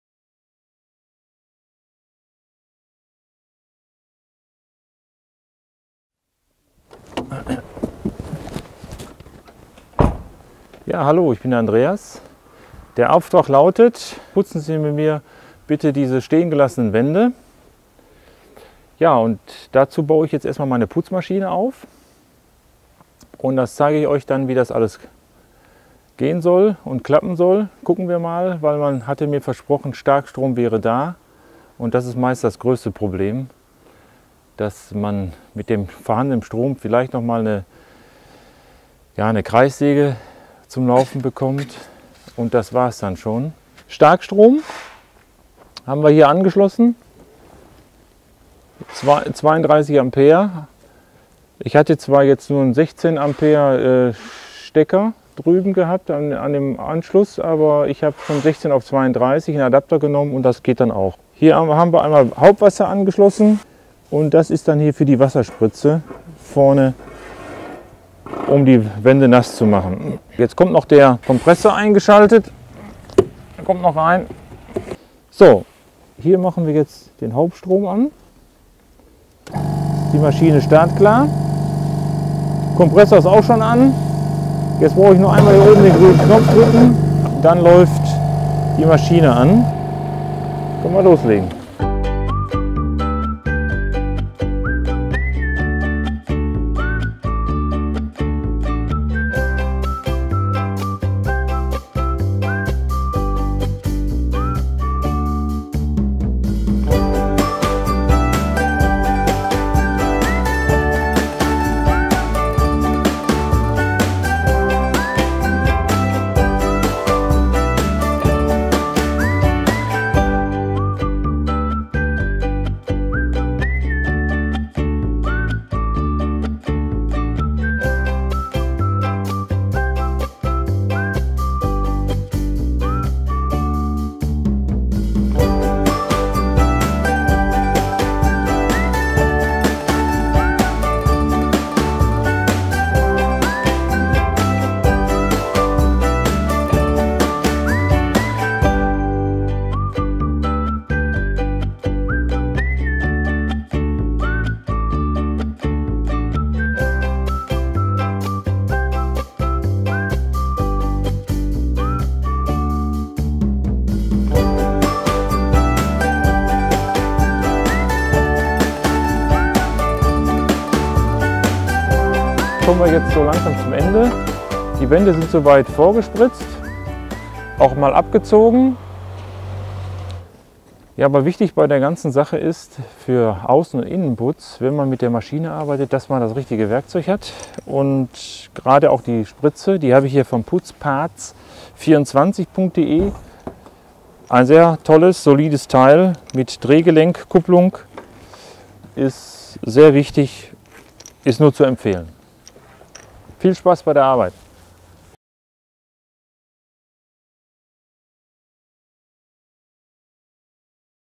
Putzparts24 Richtig verputzen mit Putzmaschine / Plastering Machine
Egal ob sie eine Putzmaschine von Putzmeister, PFT oder M-Tec verwenden, in diesem Video zeigt ihnen Andreas  an hand der VR8 von V.E.P. worauf es beim arbeiten mit einer Putzmaschine ankommt.

Ersatzteile und Maschinen können sie jederzeit bequem in unserem Online Shop bestellen, unter